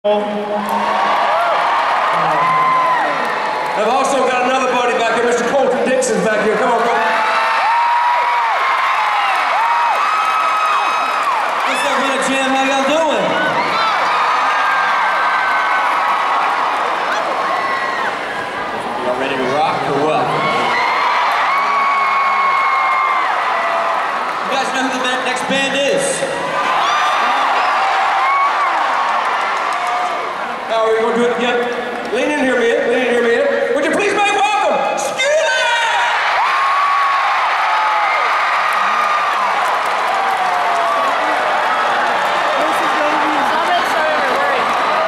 Introducing Skillet at Winter Jam 2015
Randy Armstrong, Anthony Armstrong (RED), and Colton Dixon announcing Skillet as the next band to perform at Winter Jam 2015.